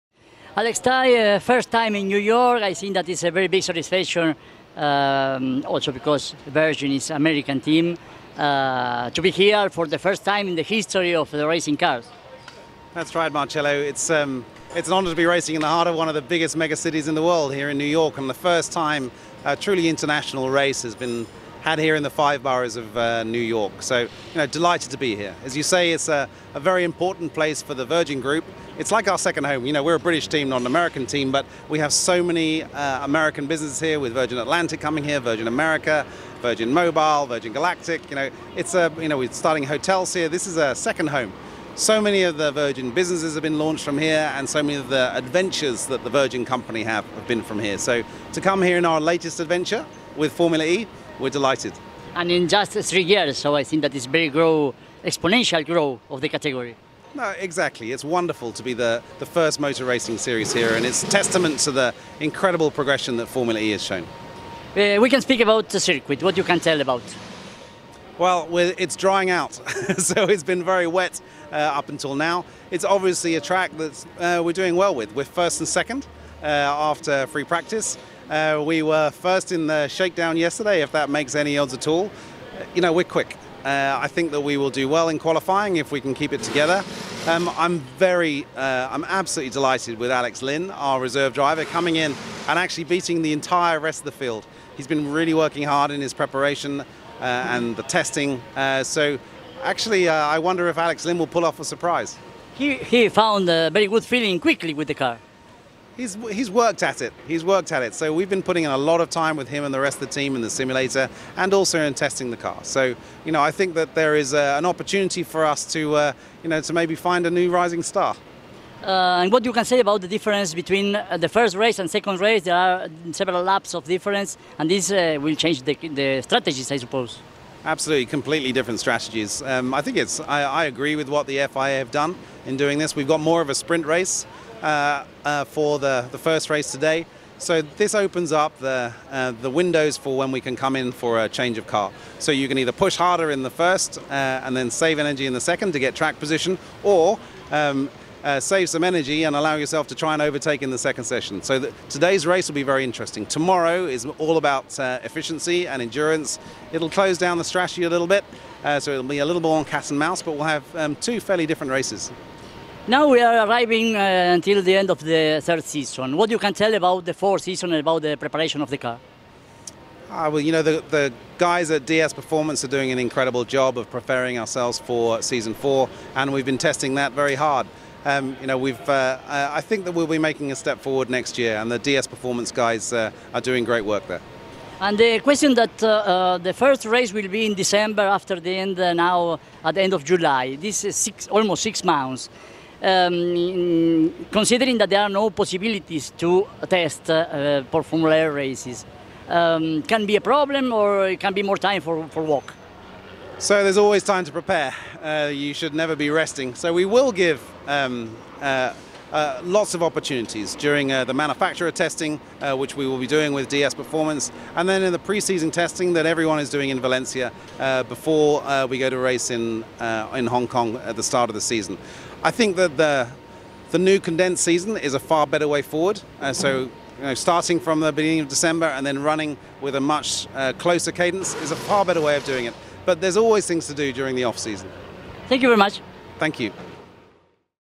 L’ottimismo di Alex Tai - Electric Motor News a New York
Il Team Principal del DS Virgin Racing, Alex Tai, è molto ottimista in modo particolare dopo il risultato della prima prova libera a New York, nella quale Alex Lynn ha segnato il miglior tempo seguito da Sam Bird.
Oltre l’ottimismo si è parlato di Formula E e della vettura per la prossima stagione.
Intervista di Marcelo Padin, riprese di Daniele Drago da New York City.